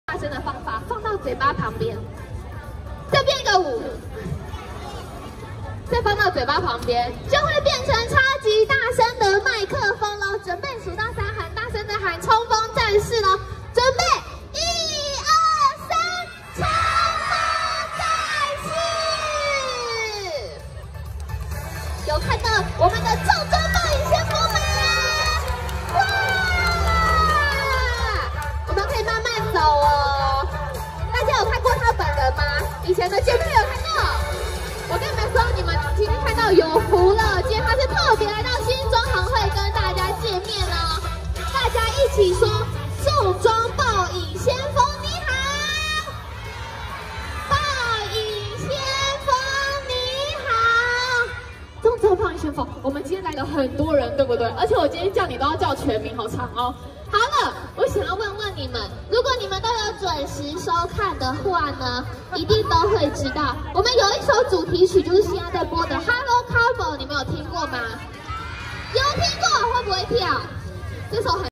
豪宅玩具~2709~2023新北市 新莊 宏匯廣場 Hello Carbot衝鋒戰士 衝鋒戰士重裝爆影先鋒見面會 登場
新莊 宏匯廣場 6/24   Hello Carbot衝鋒戰士 衝鋒戰士重裝爆影先鋒見面會